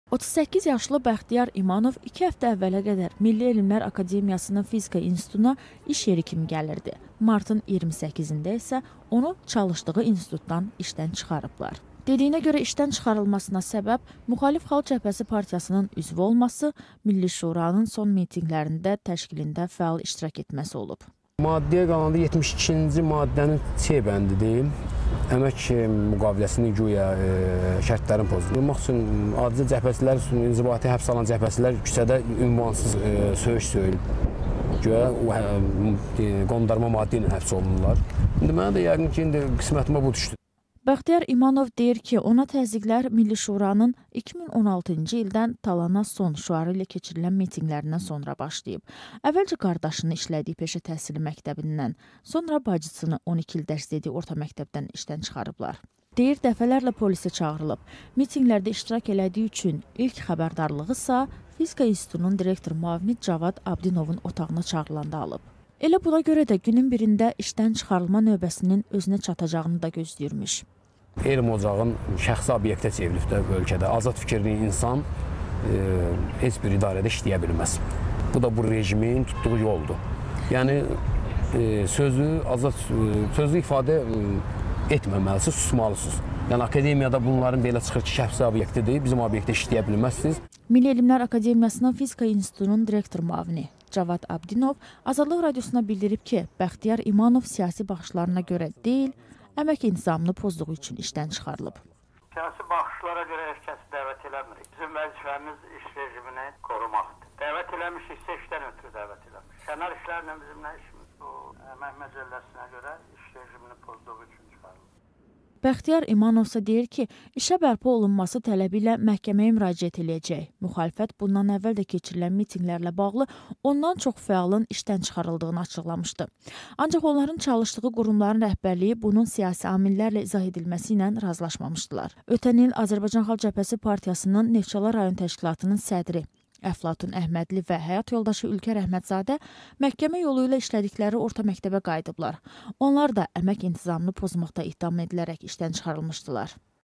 Cəbhəçi fəal: 'Məni mitinqə görə işdən çıxarıblar'
Fizika İnstitutu isə bu iddiaları qəbul etmir.